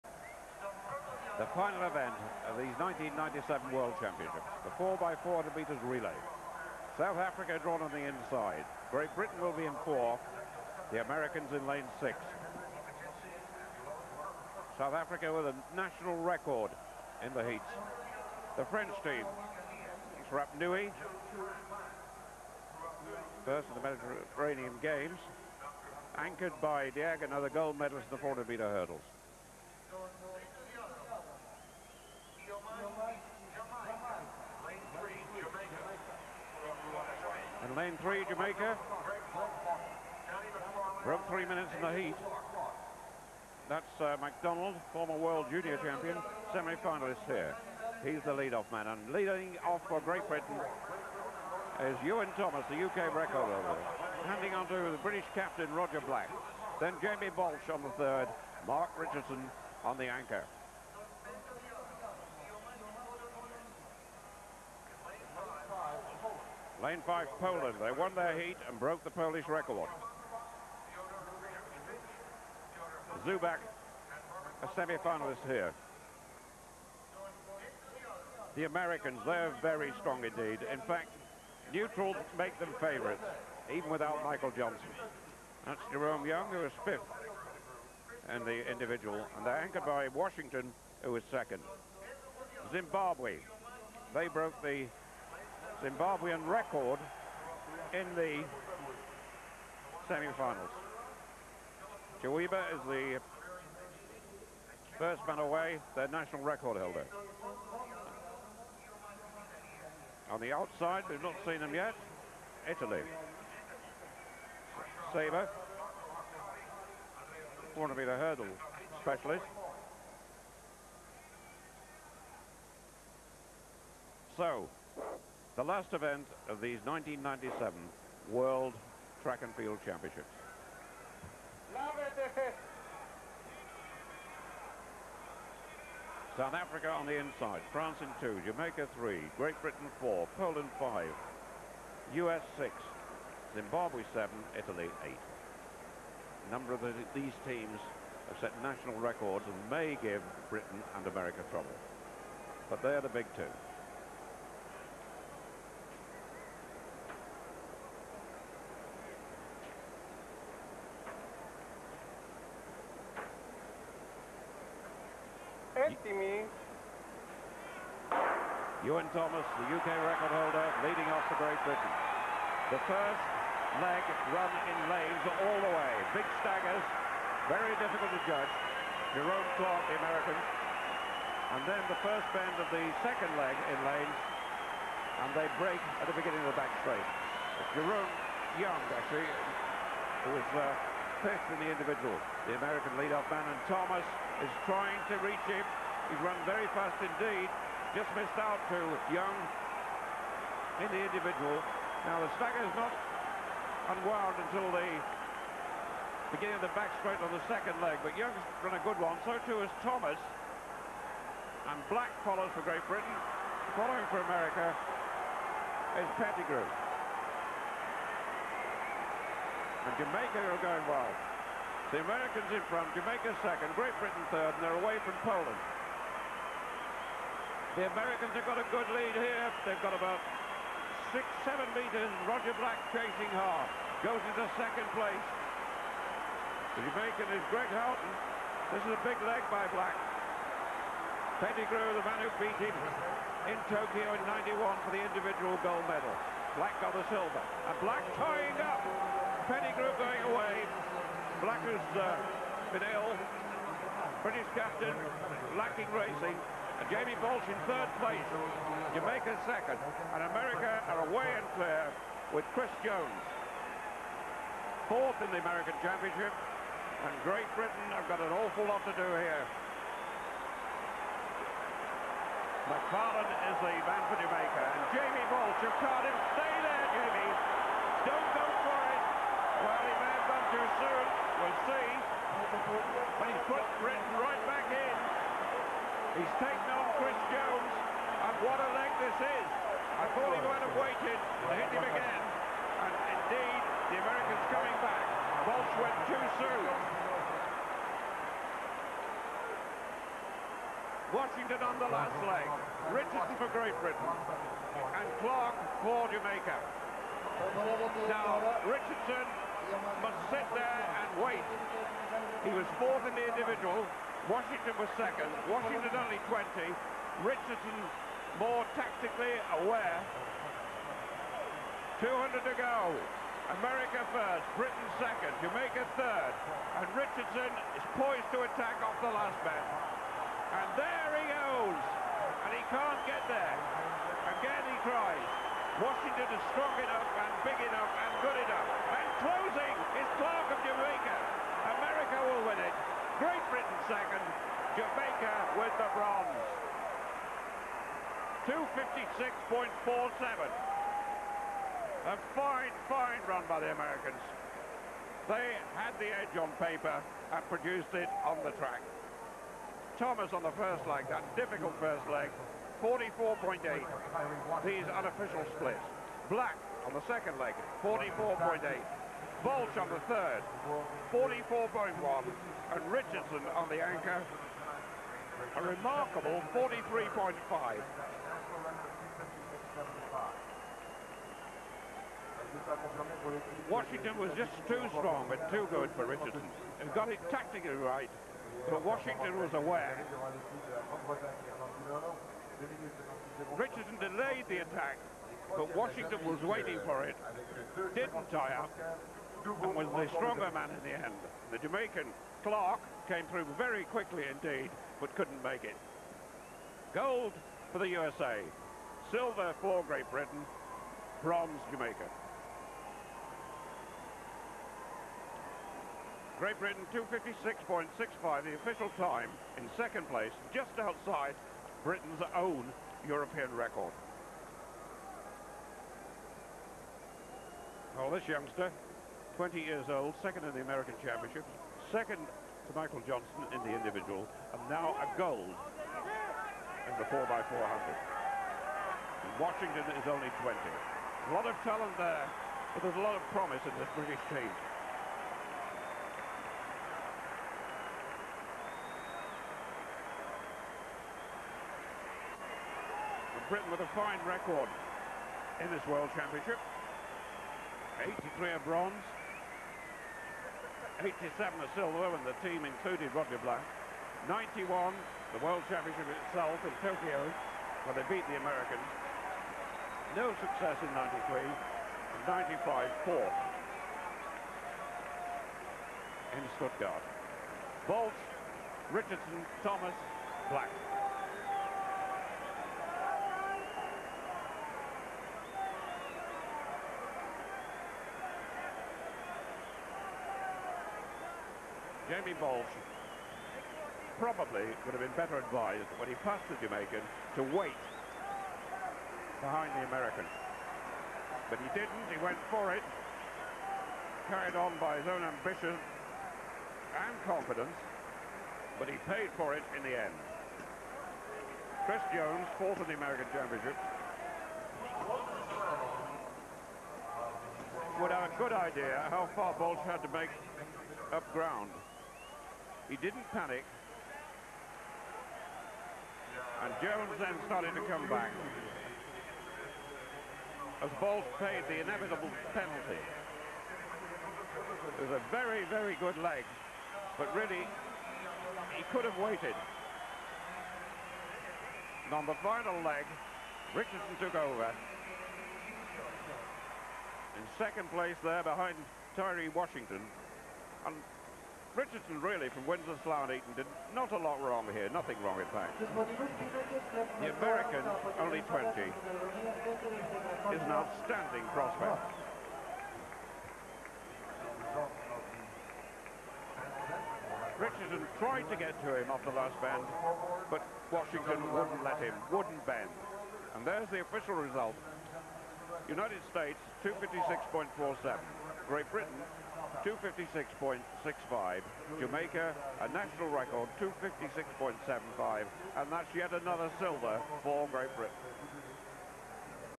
7767 World Track and Field 1997 4x400m Men
1997 World Athens 4x400m Men
Buying something on Amazon
1 Great Britain Iwan Thomas, Roger Black, Jamie Baulch, Mark Richardson 2:56.65
2 Jamaica Michael McDonald, Gregory Haughton, Danny McFarlane, Davian Clarke 2:56.75 NR
3 Poland Tomasz Czubak, Piotr Rysiukiewicz, Piotr Haczek, Robert Maćkowiak 3:00.26
4  South Africa Arnaud Malherbe, Hezekiel Sepeng, Hendrik Mokganyetsi, Llewellyn Herbert 3:00.26 NR
5  France Jean-Louis Rapnouil, Marc Foucan, Fred Mango, Stephane Diagana 3:01.07
6  Zimbabwe Tawanda Chiwira, Phillip Mukomana, Savieri Ngidhi, Ken Harnden 3:01.43
7  Italy Ashraf Saber, Marco Vaccari, Andrea Nuti, Fabrizio Mori 3:01.52
DSQ  United States Jerome Young, Antonio Pettigrew, Chris Jones, Tyree Washington 2:56.47